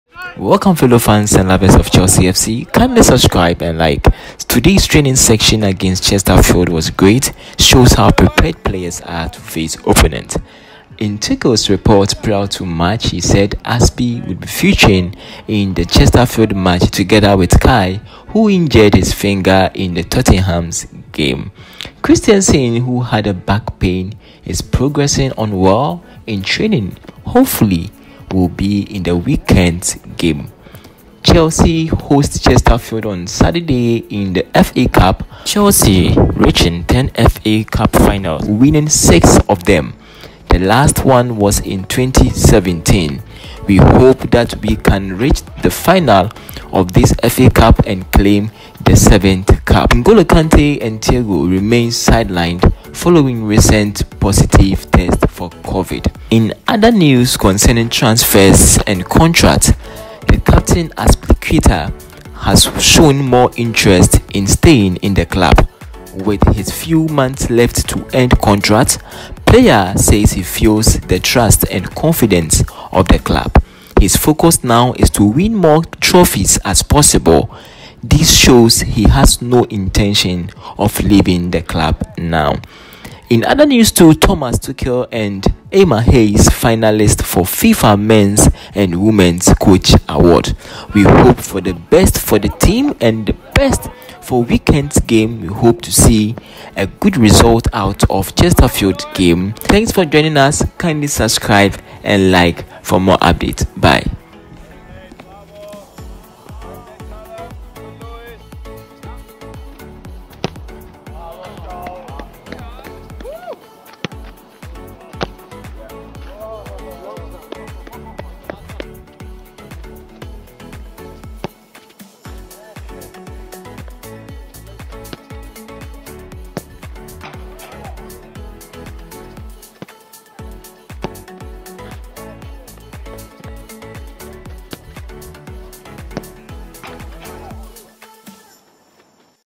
🔥Training section || Chelsea vs Chesterfields || Azpi contract intention|| Tuchel to win another …
Azpilicueta & Havertz Both Available For FA cup Third Round Clash

todays training section against Chester field was great,  shows how prepared players are to face opponent. I Tuchel report prior to match

Azpi would be featuring in the Chesterfield match togther with Kai who injured his finger in the Tottenhams game.
Christensen who hard a back pain is progressing on well in training, hopefully will be in weekends game.
Chelsea host Chesterfield  Saturday in the FA cup, Chelsea have reached 10 FA cup final winning  six of them, the last was in 2017
Ngolo Kante and Thiago remained sidelined following recent positive test for cover

The captain Azpilicueta , has shown more interest in staying in the club , with his few month left to end contract, Player feels the trust and confidence of the club. His focus now is to win more trophies as possible. This show he has no intention of leaving now.

Thomas Tuchel and and Emma Hayes finalist for FIFA Men’s and Women’d Coach awards.